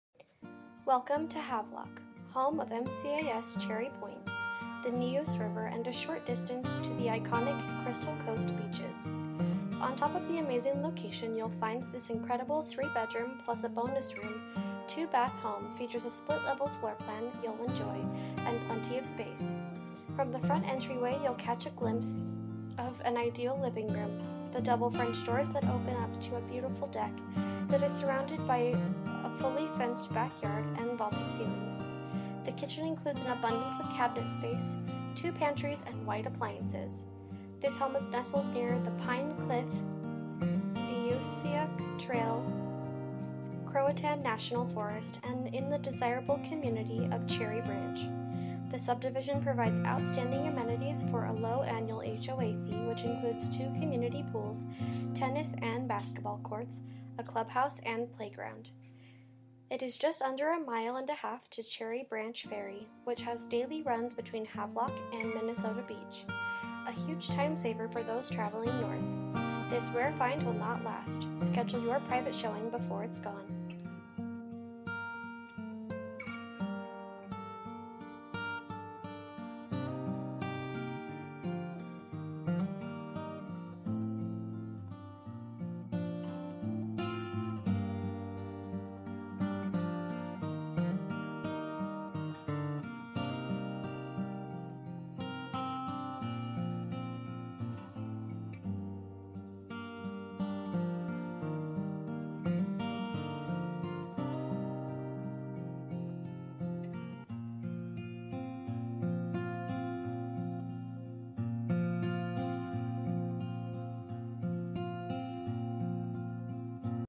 Homes for sale - 121 Quarter Horse, Havelock, NC 28532
Welcome to Havelock, home of MCAS Cherry Point, the Neuse River and a short distance to the iconic Crystal Coast Beaches. On top of the amazing location, you'll find this incredible 3 bedroom plus a bonus room, 2 bath home, features a split level floor plan you'll enjoy and plenty of space. From the front entry way, you'll catch a glimpse of an ideal living room, the double french doors that open up to a beautiful deck that is surrounded by a fully fenced backyard and vaulted ceilings. The kitchen includes an abundance of cabinet space, 2 pantries, and white appliances. This home is nestled near the Pine Cliff Neusiok Trail Croatan National Forest and in the desirable community of Cherry Branch. The subdivision provides outstanding amenities for a low annual HOA fee which include 2 community pools,  tennis and basketball courts, a clubhouse and playground. It is also just under a a mile and a half to the Cherry Branch Ferry which has daily runs between Havelock and Minnesota Beach, a huge time saver for those traveling North. This rare find will not last, schedule you private showing before it's gone.

Bedrooms: 3
Bathrooms: 2
Square Feet: 1809
Price: $189,900

For more information about this property, please contact Frost Real Estate Team at or . You can also text 5880300 to 67299.

11/01/2019 01:31:01 pm

Last modified: